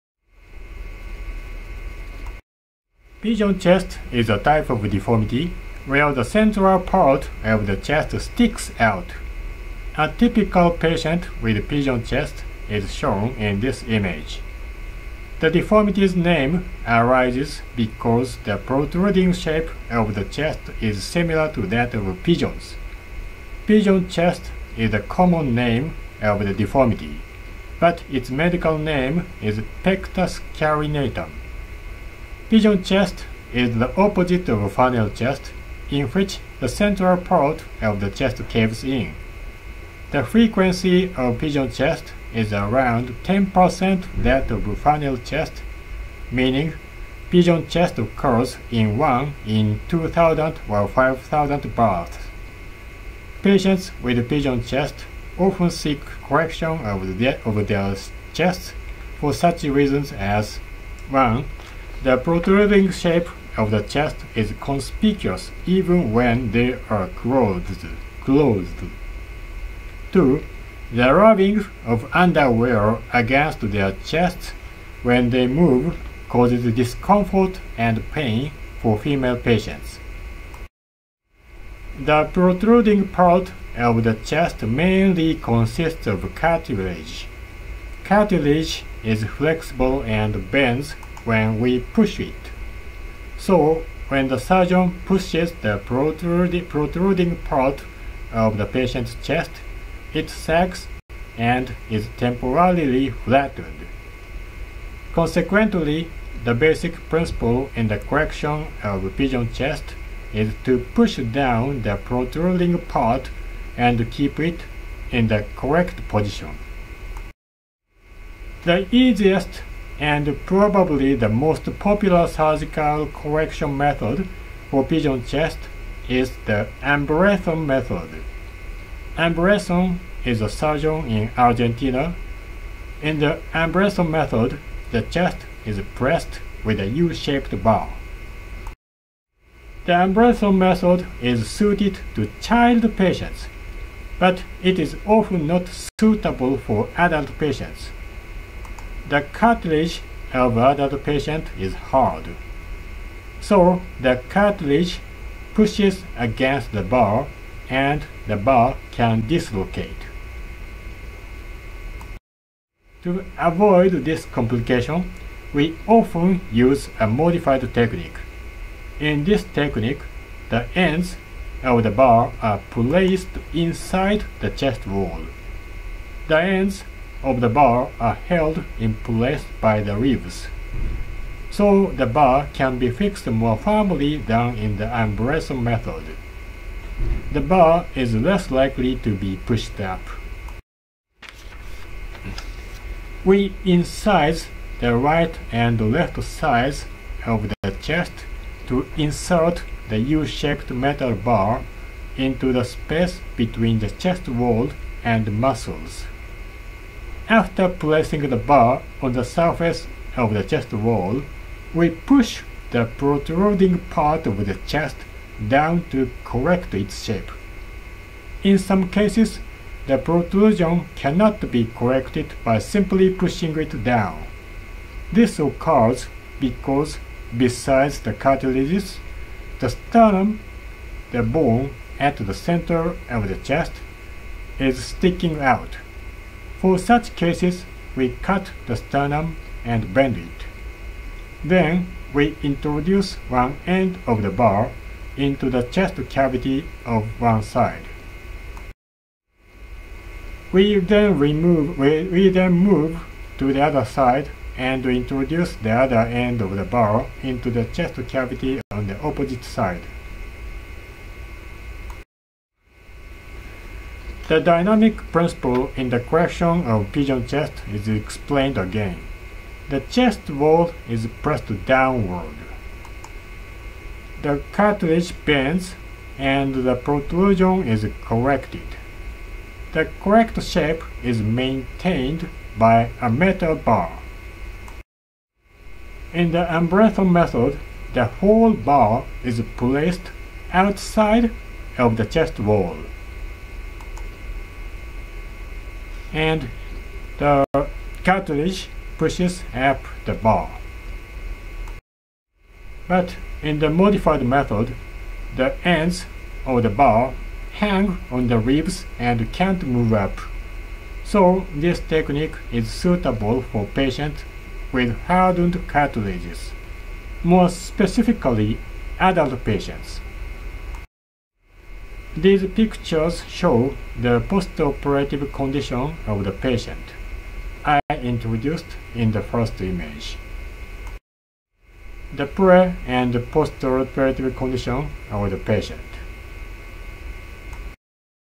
Treatment of Pigeon Chest
This short movie introduces you our modified technique to correct pigeon chest. This method is suitable for the treatment of adult patients.